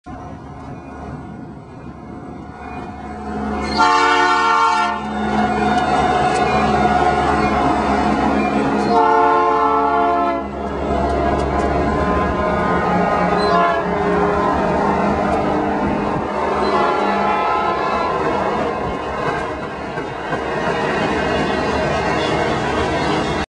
UP at Canyon
A UP train loaded with minerals from Trona comes down the Palmdale cutoff at Canyon and for more great videos and movies,